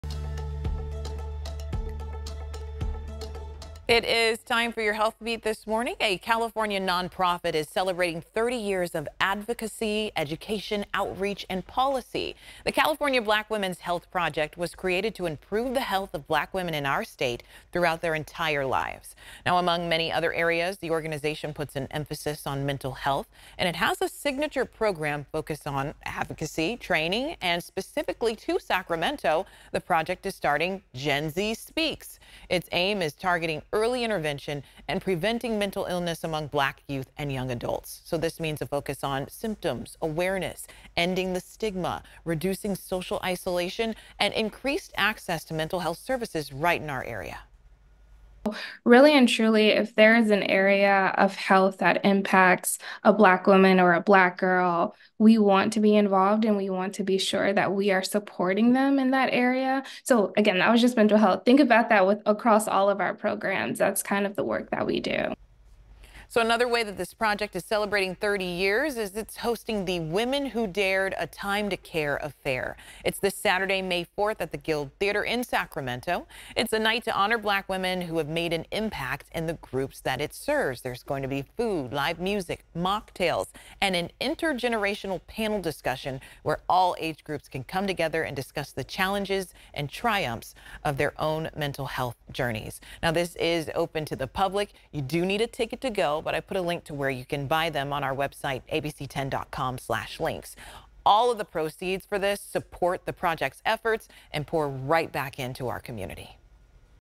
California Black Women's Health Project celebrates 30 years
California Black Women's Health Project is a non-profit organization aimed to improve the health of black women in the state from preconception through aging.